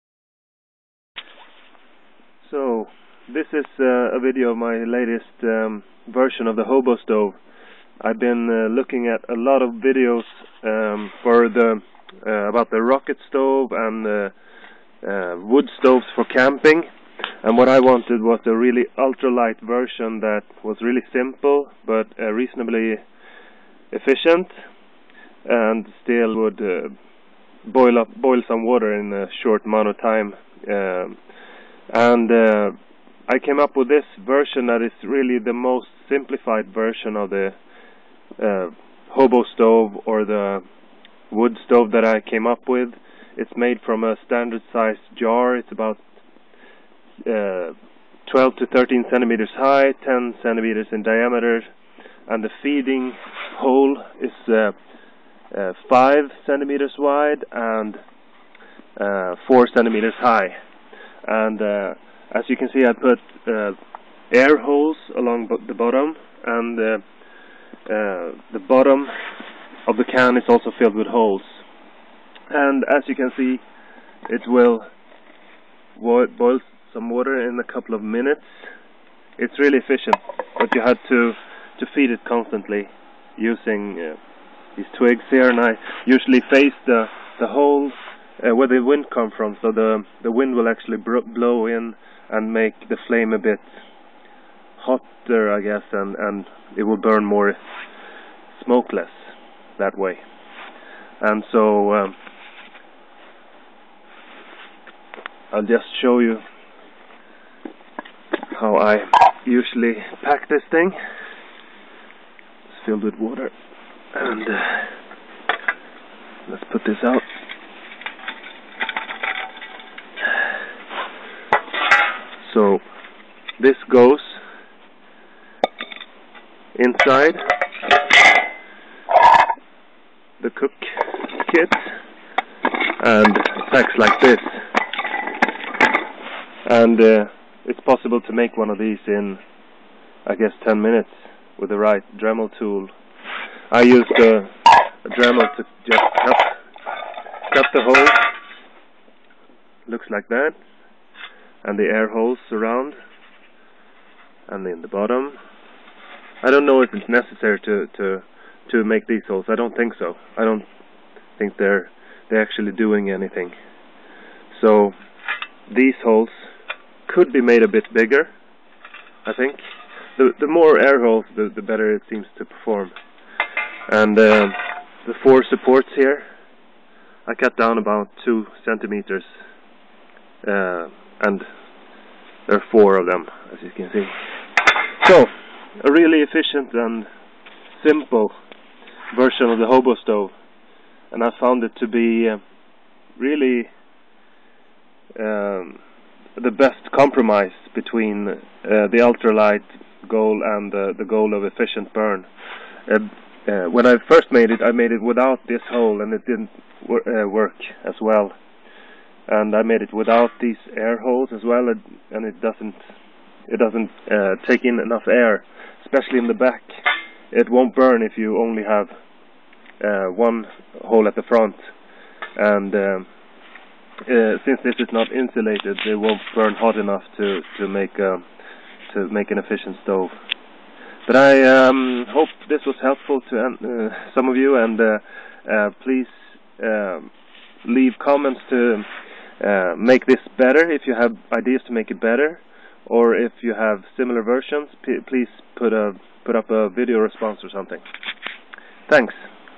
Simple Ultra-Light Wood Burning Stove
This is an ultra light version of the hobo/rocket stove. I wanted to make to most simple, inexpensive and lightweight stove possible. There are a lot of double walled stoves that work fine but I found that they are heavier and much more difficult to feed during the burn. Another downside is that they often go out and start smoking when you add fuel. This version i much more convenient.  I found that it burns quite cleanly and it is very more practical since you can feed the fire for as long as you like to cook. The single-wall design is enough for a clean burn and makes the stove truly ultra light. Please make suggestions on how the design could be improved.